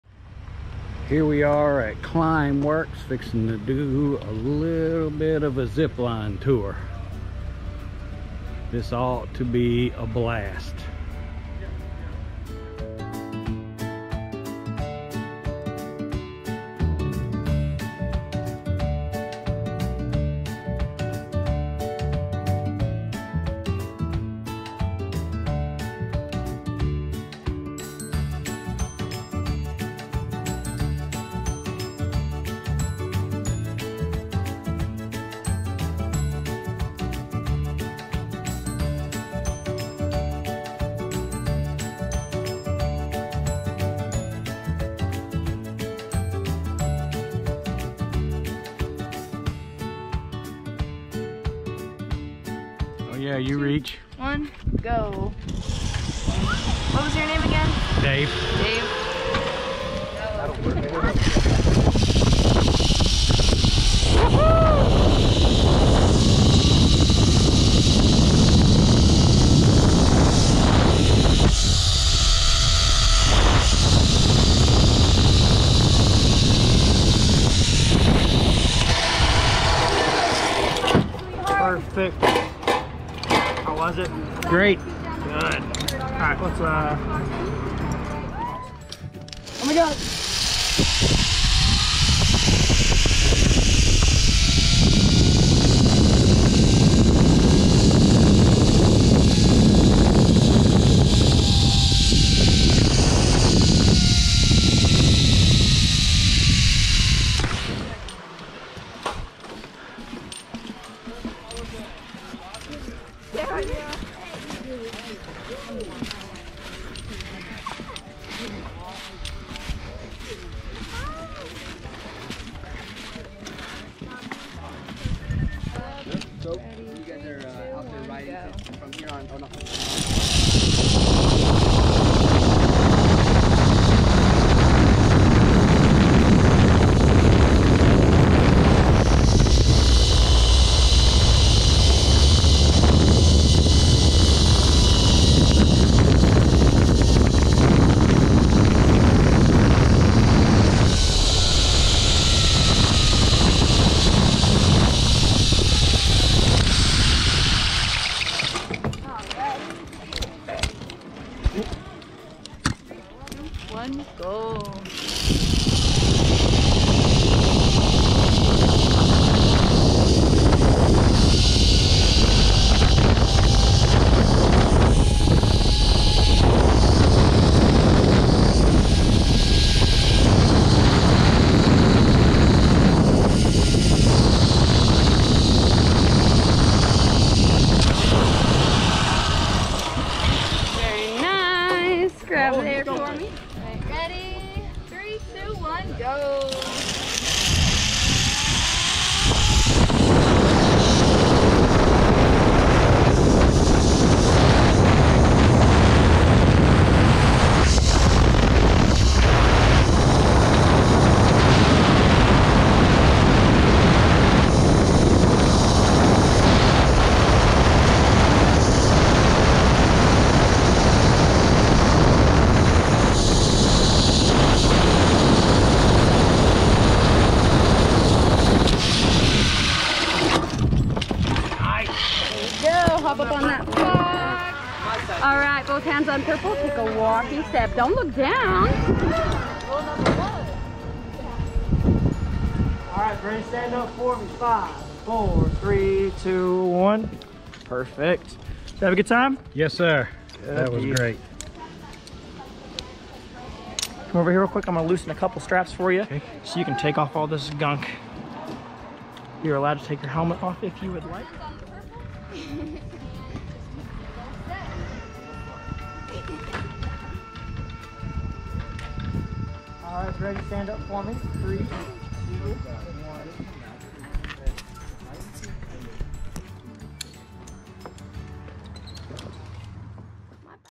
Climb Works Zip Line
Went Zip lining at Climb Works in Gatlinburg Tennessee. I will do this again as me and  my girls had a blast. The crew was awesome and a special shout out to Havener and Stevie.